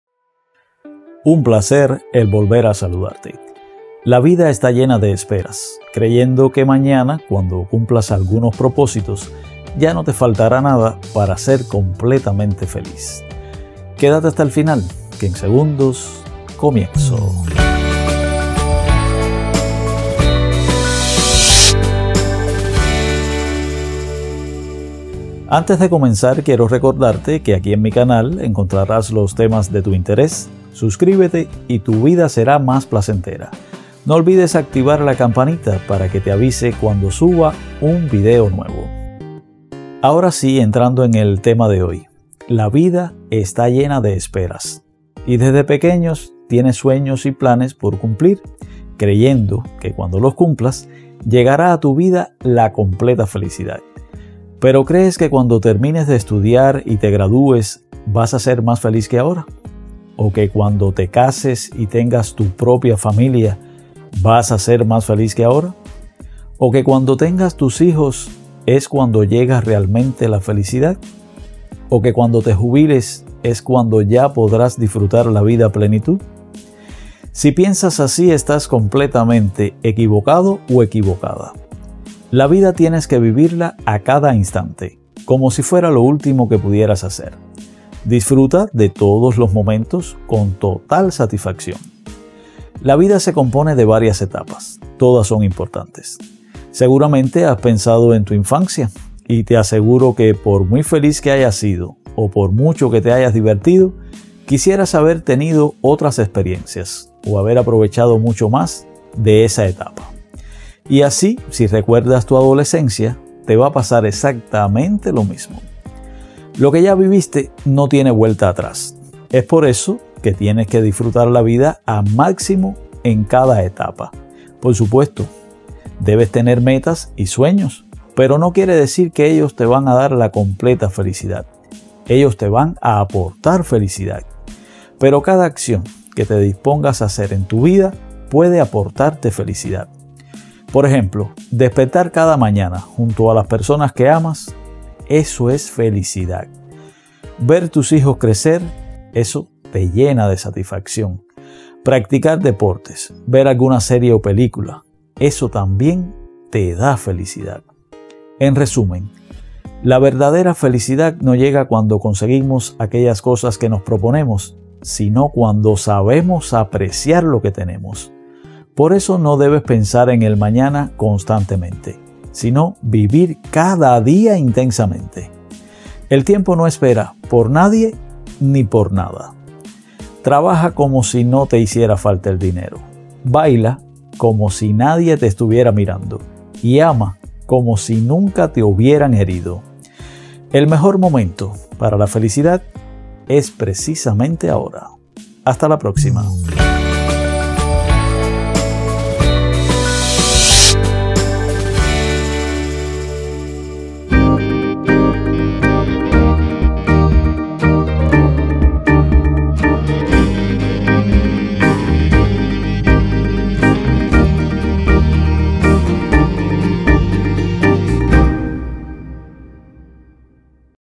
Crees que cumplir metas es sinónimo de felicidad.
👀 La verdadera felicidad no llega cuando conseguimos aquellas cosas que nos proponemos, sino cuando sabemos apreciar lo que tenemos, por eso no debes pensar en el mañana constantemente, sino vivir cada día intensamente.  (no dejes de ver el video).👍👍👍